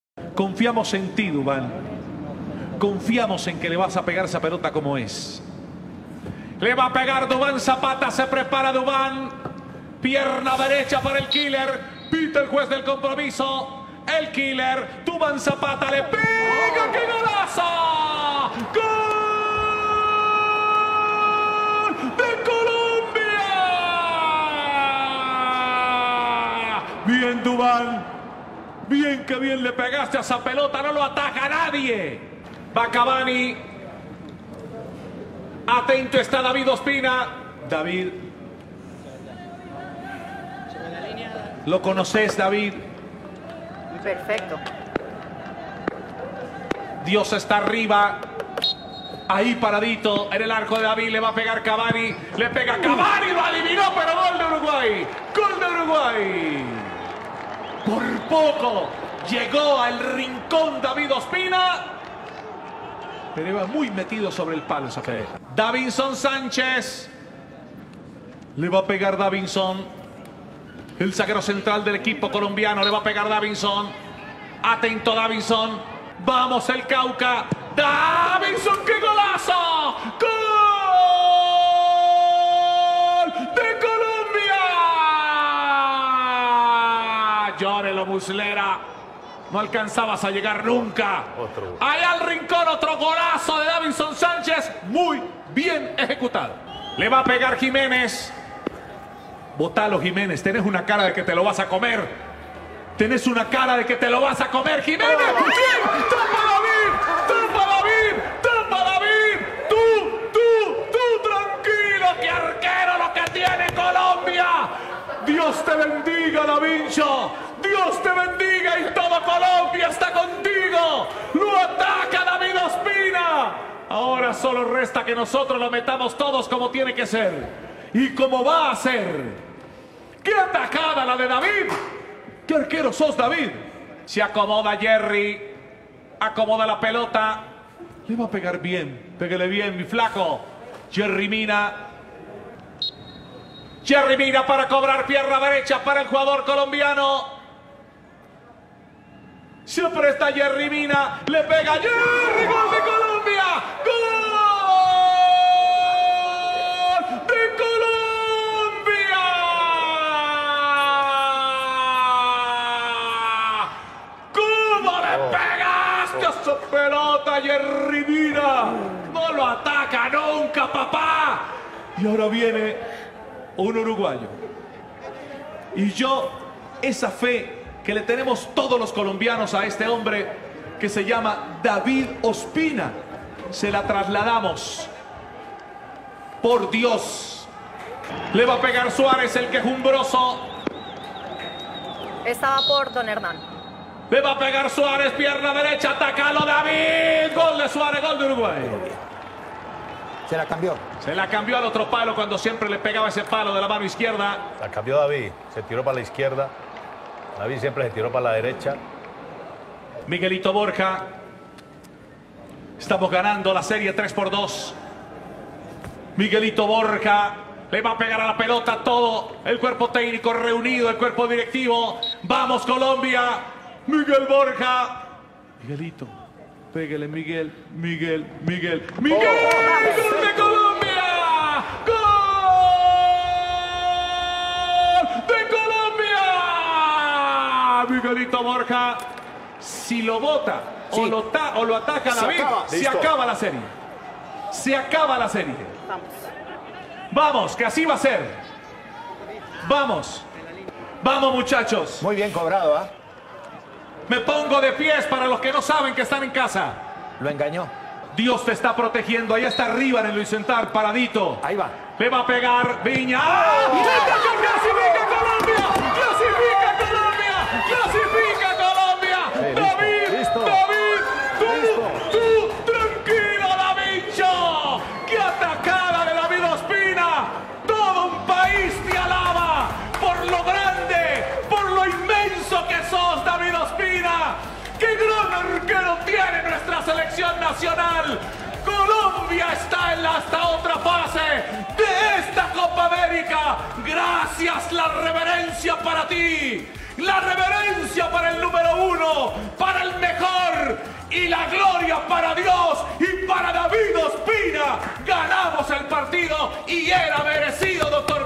Tuu!! tranquilooo!Emocionante!! narración Javier fernandez Colombia vs Uruguay (copa América 2020)
narración emocionante del cantante del gol Javier Fernández! Colombia vs Uruguay cobros desde el tiro penal por la semifinal de la copa América Brasil 2020.

 comparte ! vive la pasión del fútbol!